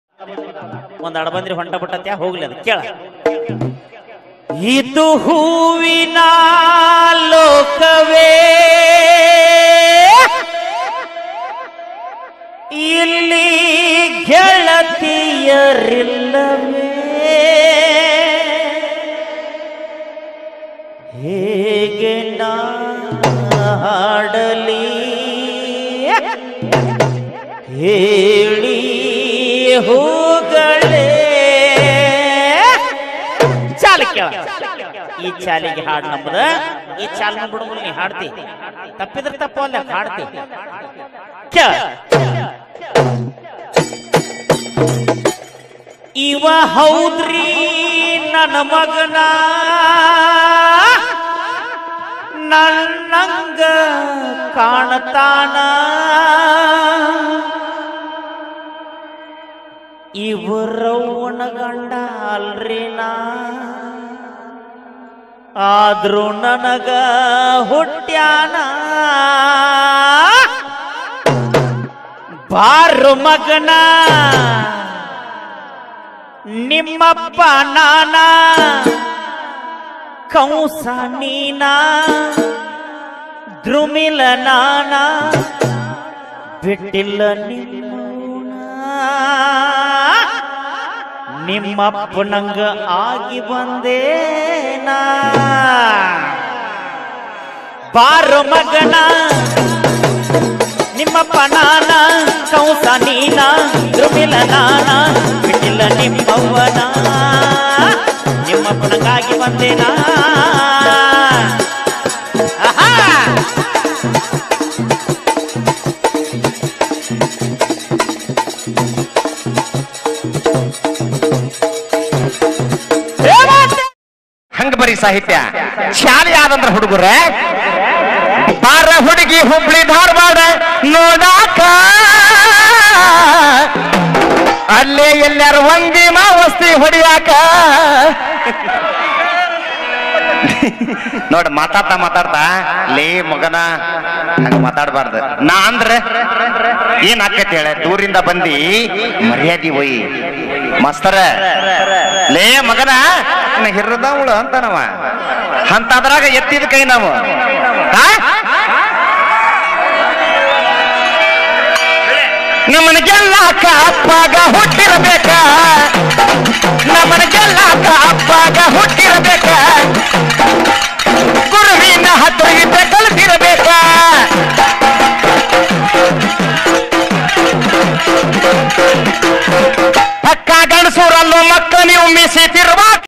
ಈಶ್ವರ್ ಮಾಸ್ತರ್ ಮಾರುತಿ ಎಚ್ ಲಮಾಣಿ ಒಂದೇ ಸ್ಕ್ರೀನ್ನಲ್ಲಿ ಆಡಿರುವ ಸವಾಲ್ ಭಜನೆ
bhajana music